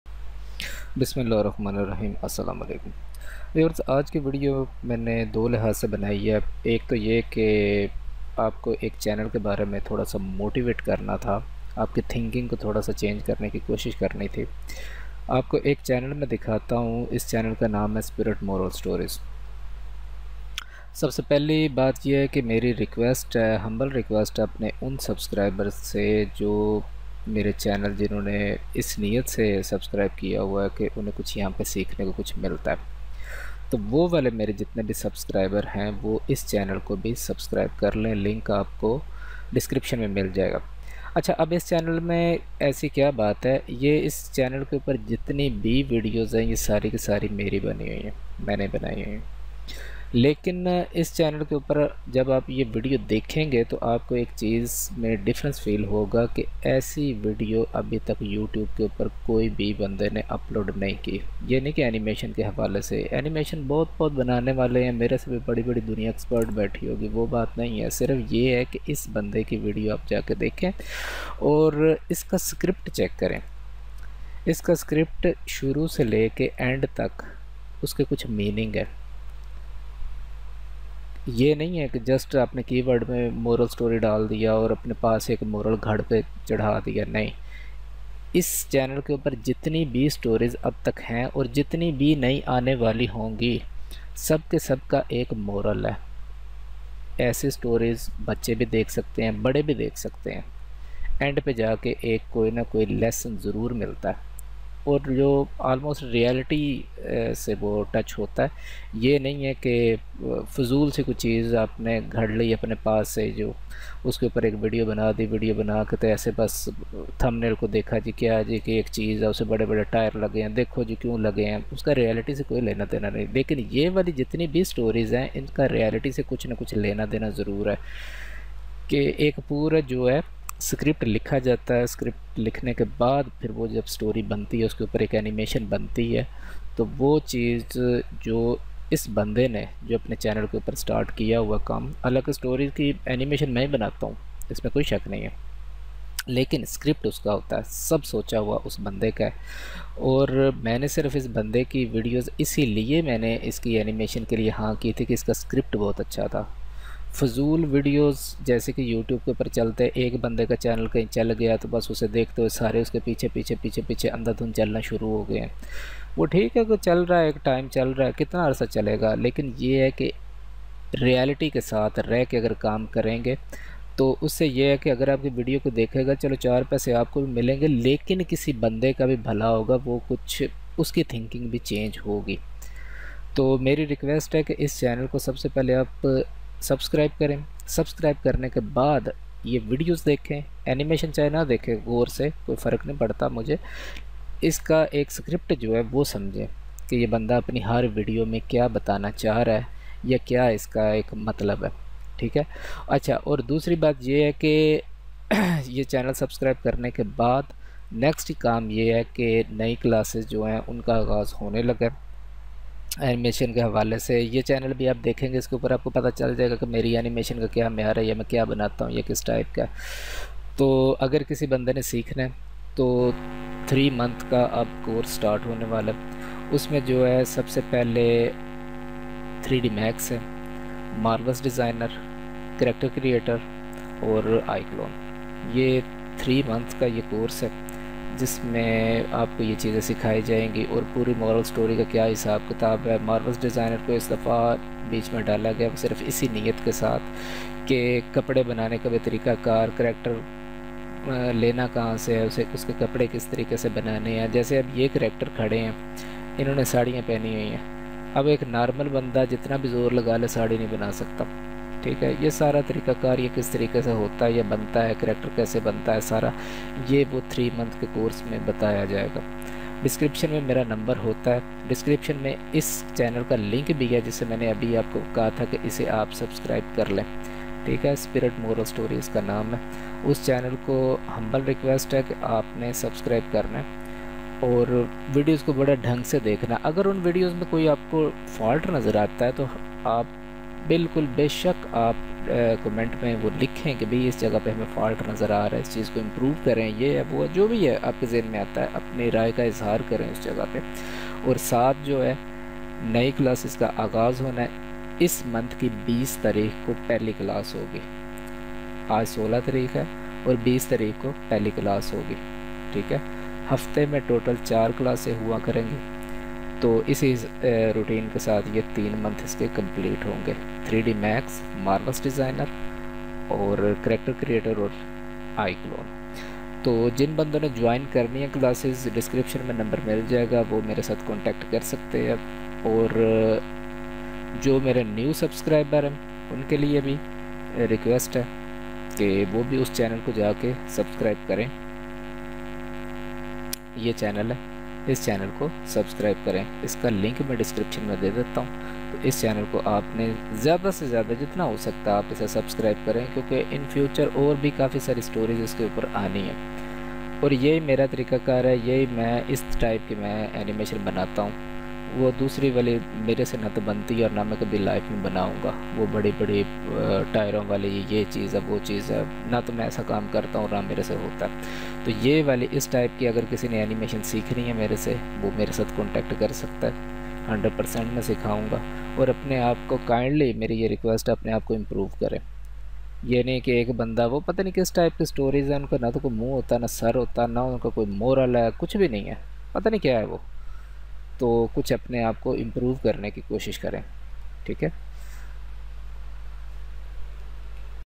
join new classes for learn professional animation | How to make moral stories in Hindi\Urdu
join new classes for learn professional animation | How to make moral stories in Hindi Urdu. In this video my new batch is ready after 3 days for learn professional animation.